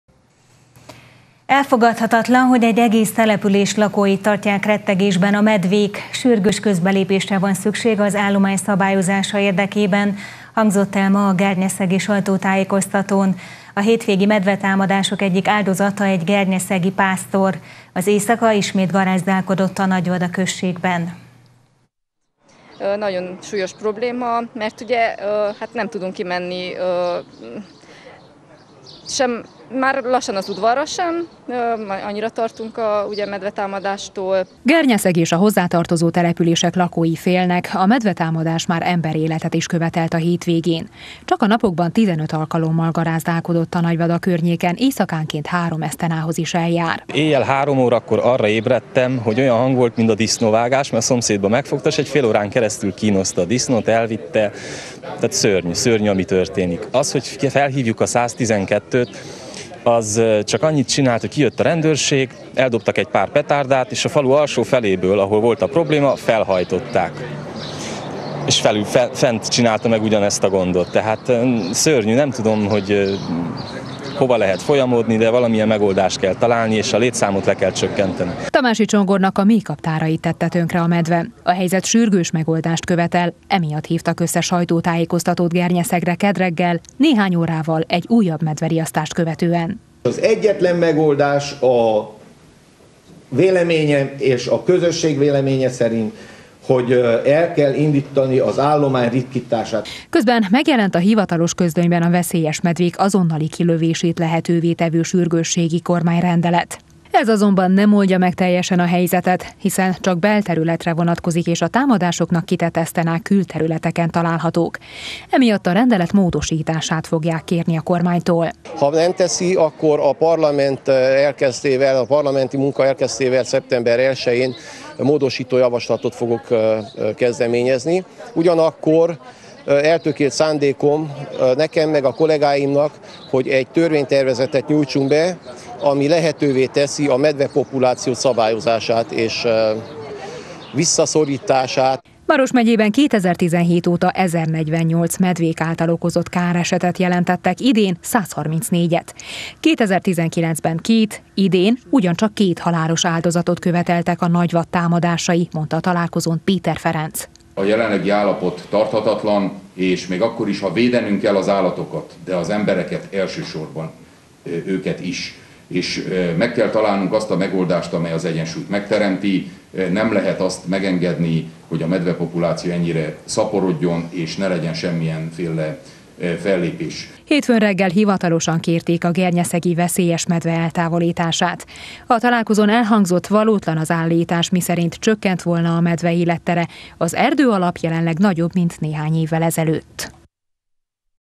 Medveveszély - ritkítani kell az állományt! – Erdélyi Magyar Televízió
Elfogadhatatlan, hogy egész települések lakóit tartják rettegésben a medvék, sürgős közbelépésre van szükség az állomány ritkítására – hangzott el a mai, gernyeszegi sajtótájékoztatón. Hétvégén két emberáldozatot követeltek a medvetámadások.